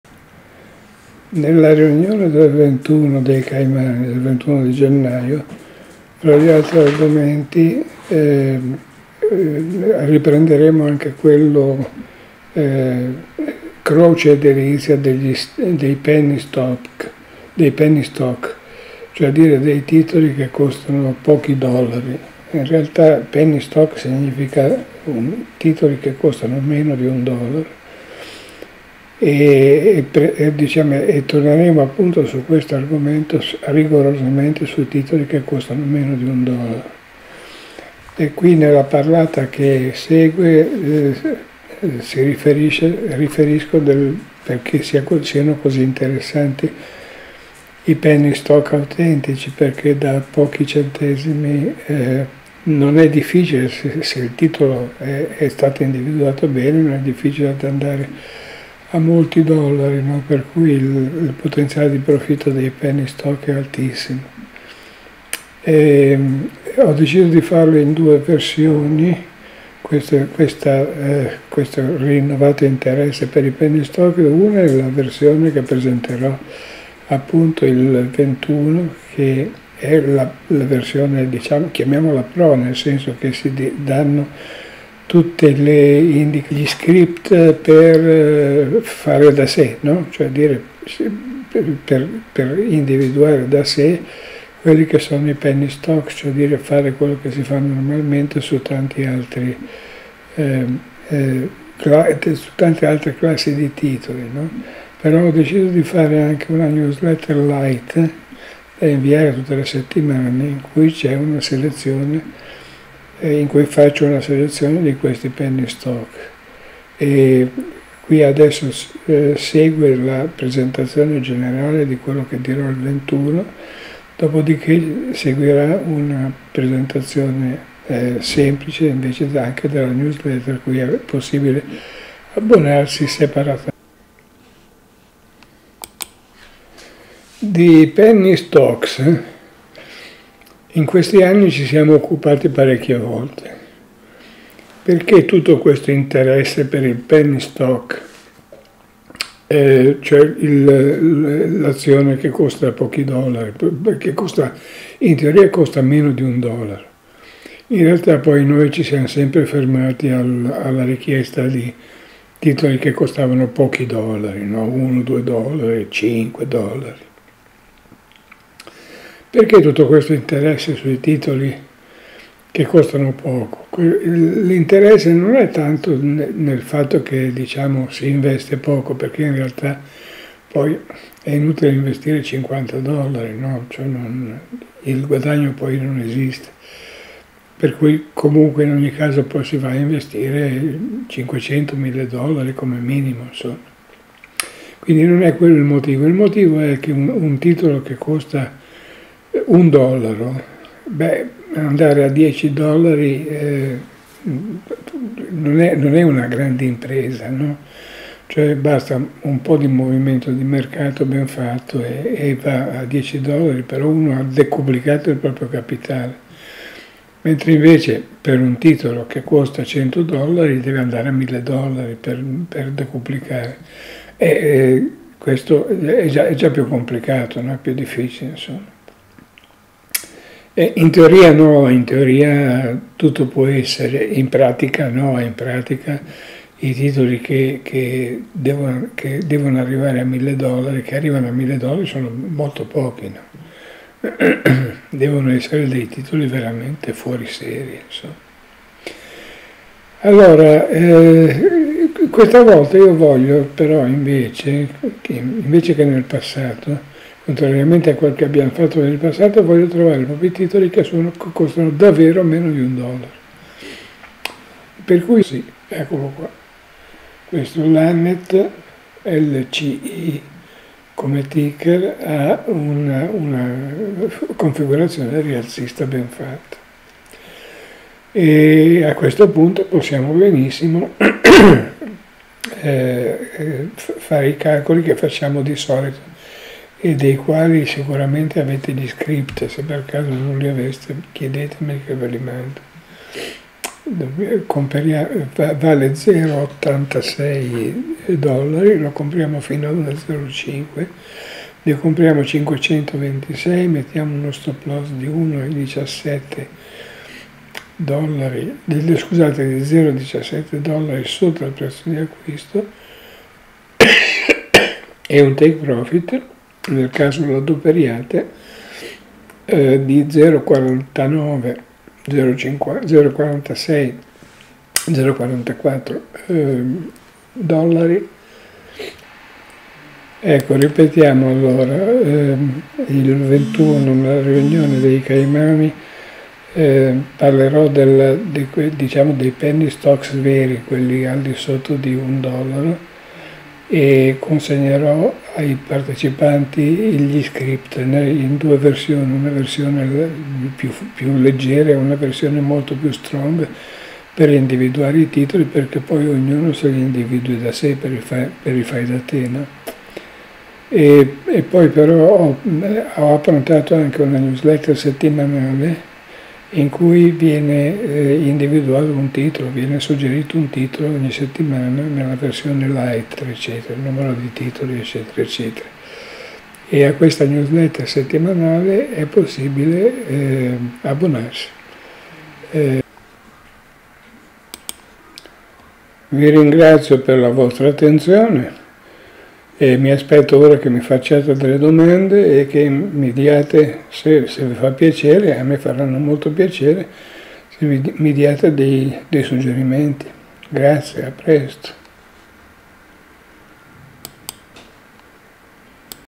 Incursione nel mondo dei Penny stocks in Borsa
Comperare un titolo a pochi centesimi di dollaro ha sempre esercitato un fascino immenso su di me